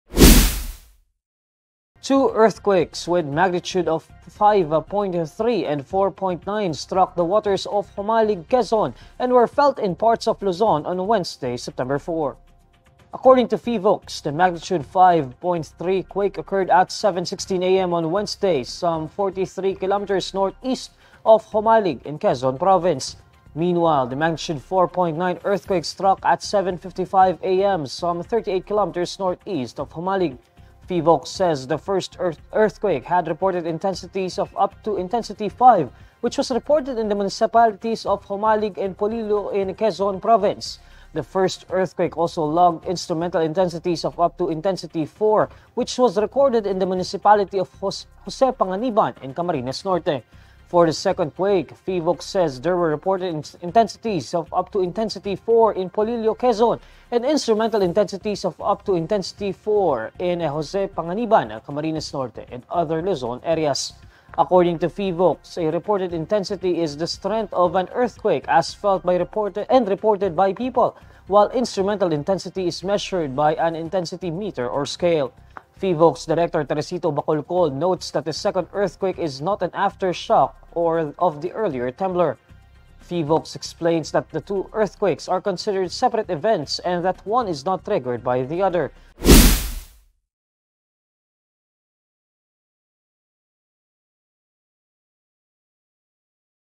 Magnitude 5.3, 4.9 earthquakes strike off Jomalig, Quezon | INQToday
Two earthquakes with magnitudes 5.3 and 4.9 strike the waters off Jomalig, Quezon, jolting parts of Luzon Wednesday morning, Sept. 4.